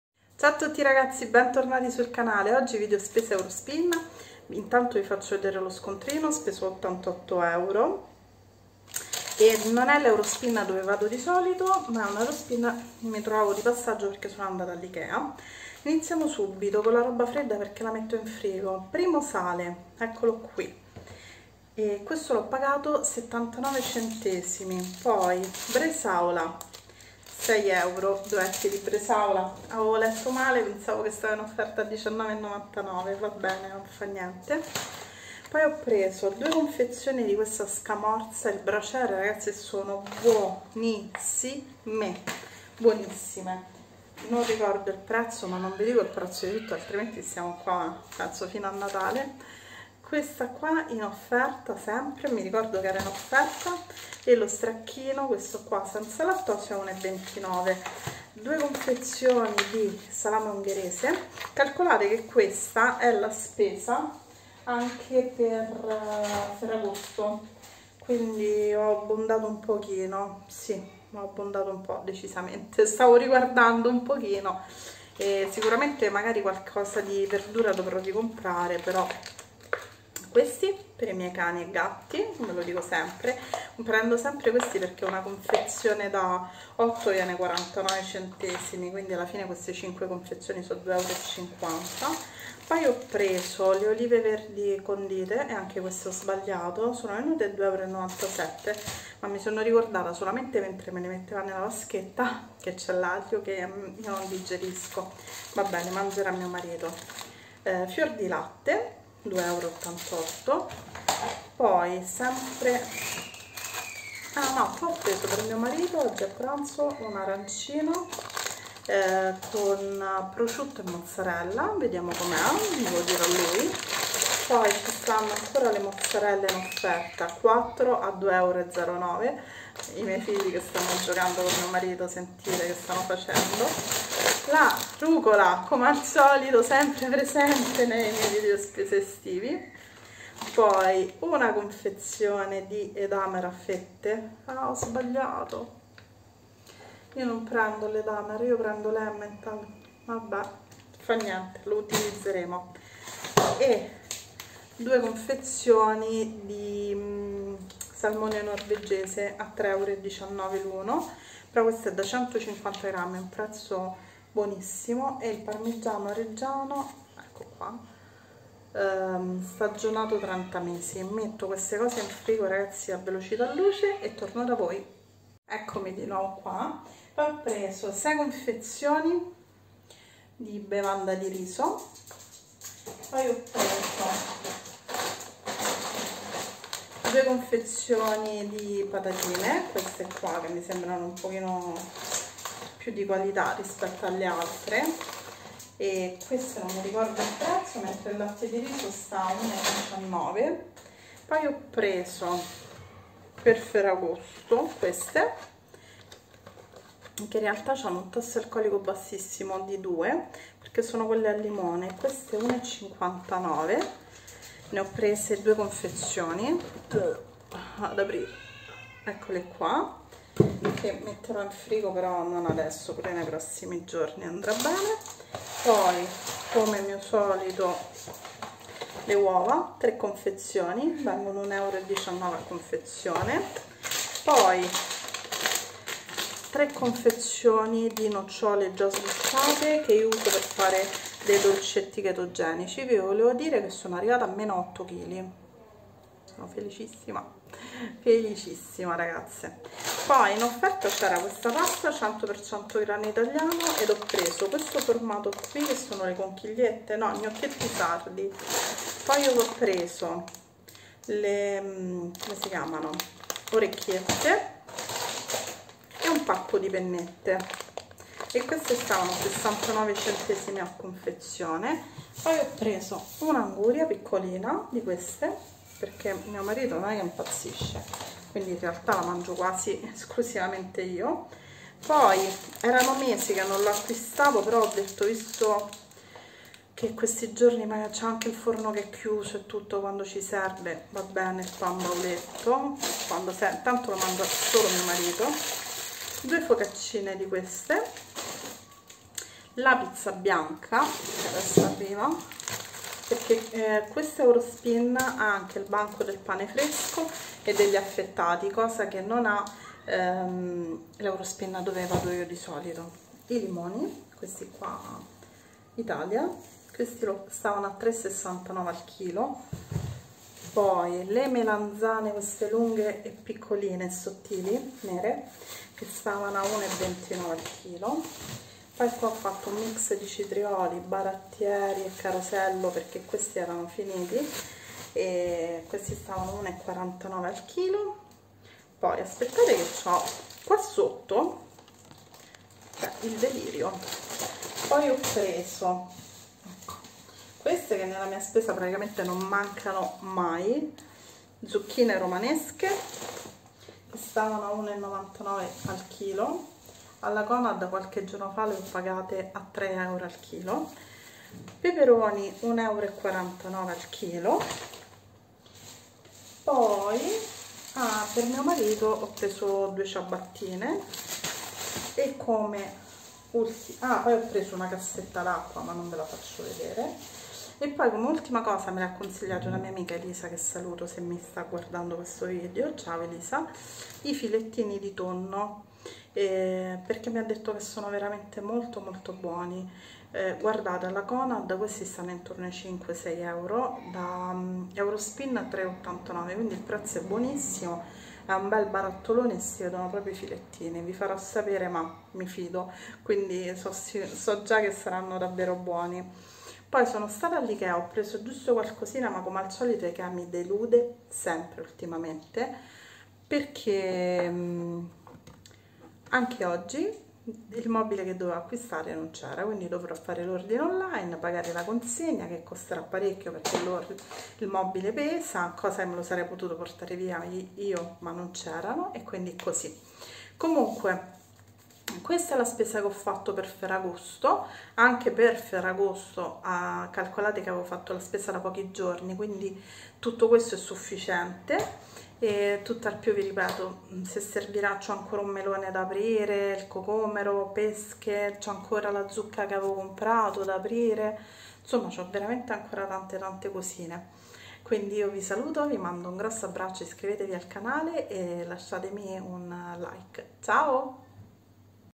VI DICO QUANTI CHILI HO PERSO... svuota spesa eurospin
ciao ragazze oggi video vi svelo quanto kg ho perso e svuoto con voi spesa Eurospin per ferragosto 🍉

per il frullatore Aigostar direttamente sul loro sito